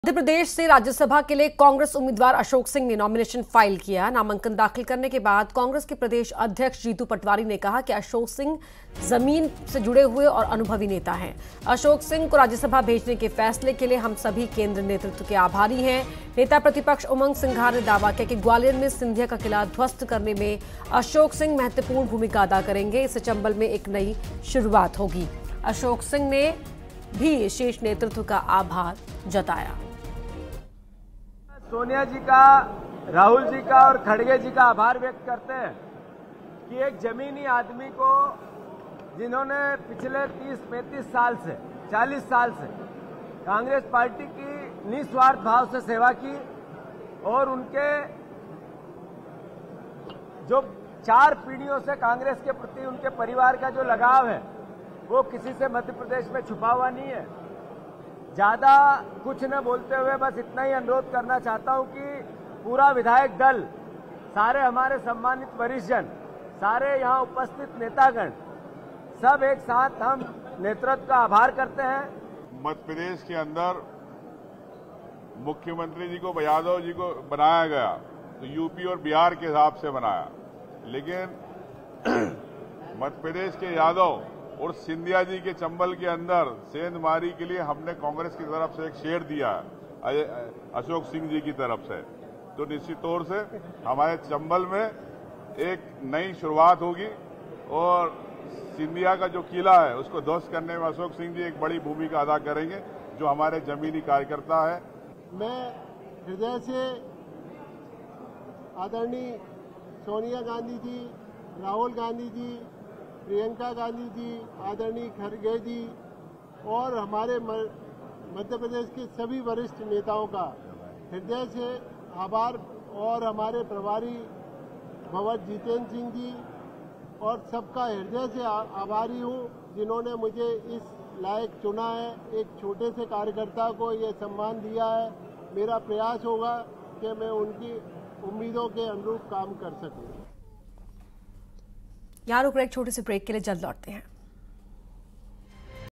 Congress नेता Ashok Singh ने भरा Rajya Sabha के लिए पर्चा, Jitu Patwari ने कही बड़ी बात | Top News
Congress नेता Ashok Singh ने भरा Rajya Sabha के लिए पर्चा, Jitu Patwari ने कही बड़ी बात |